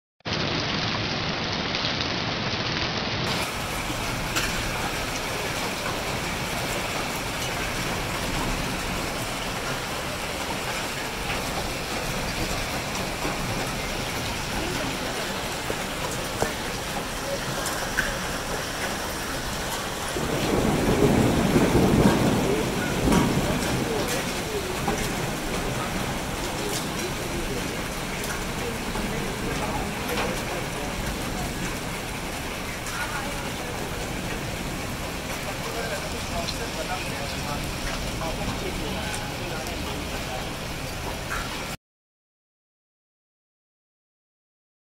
HAILSTORM IN NOIDA | 30-Apr-2020 | ஆலங்கட்டி மழை| Nature
Today we received beautiful hailstorm for few minutes. It was so good to experience along with the climate and weather conditions.

இன்று நாங்கள் சில நிமிடங்களுக்கு அழகான ஆலங்கட்டி மழையைப் பெற்றோம். காலநிலை மற்றும் வானிலை நிலைமைகளுடன் அனுபவிப்பது மிகவும் நன்றாக இருந்தது.

Twitter.  -     @leaveurnutshell